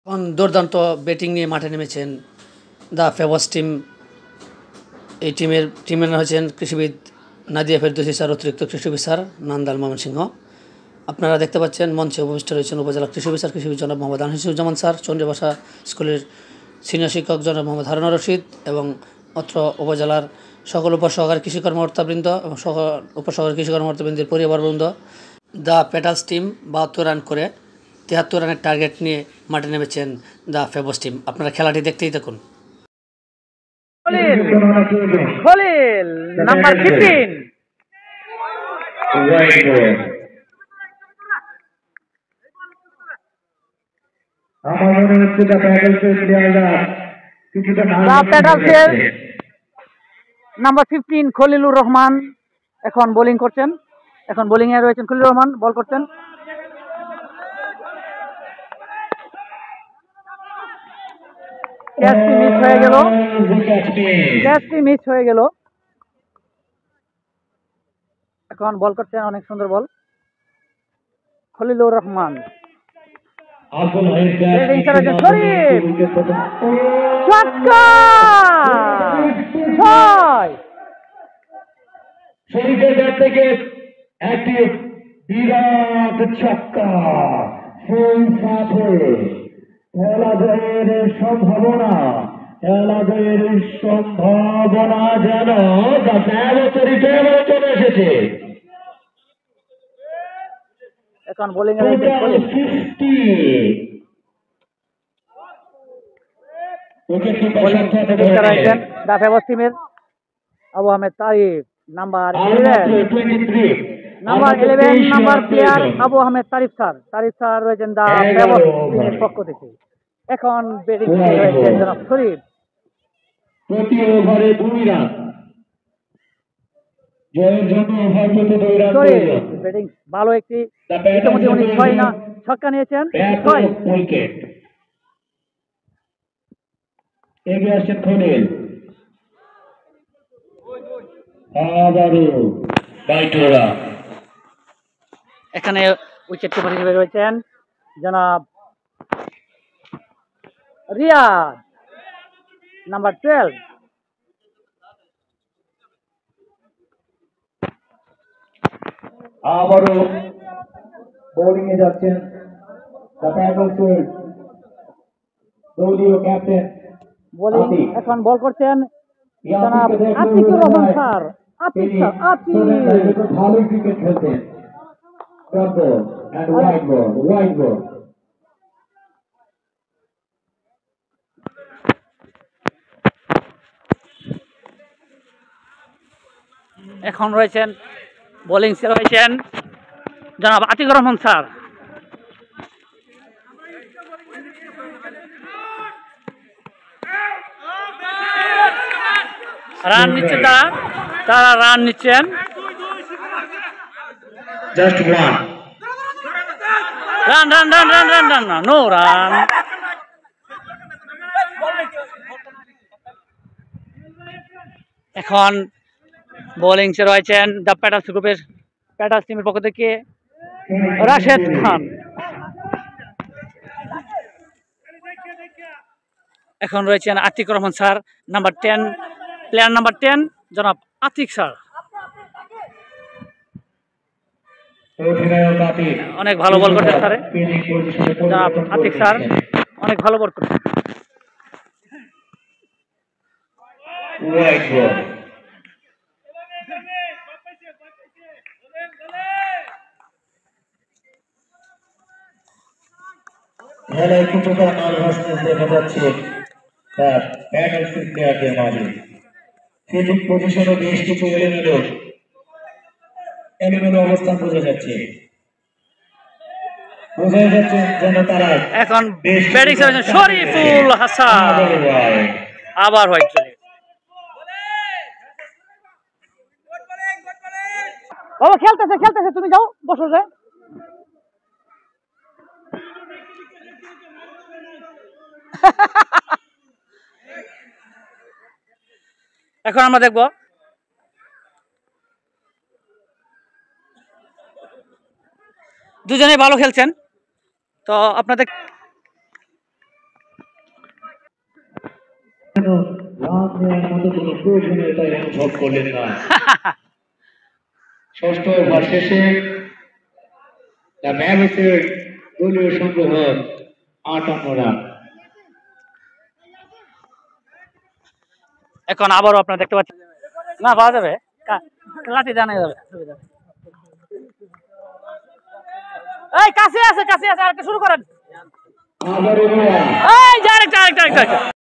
প্রীতি ক্রিকেট ম্যাচ-২০২৪ দ্যা ফেবোস বনাম দ্যা প্যাটালস্ টীম | উপজেলা কৃষি অফিস নান্দাইল ময়মনসিংহ
প্রীতি ক্রিকেট ম্যাচ-২০২৪, উপজেলা কৃষি অফিস নান্দাইল ময়মনসিংহ। আয়োজক : কৃষিবিদ মোহাম্মদ আনিসুজ্জামান উপজেলা কৃষি অফিসার নান্দাইল ময়মনসিংহ মহোদয়।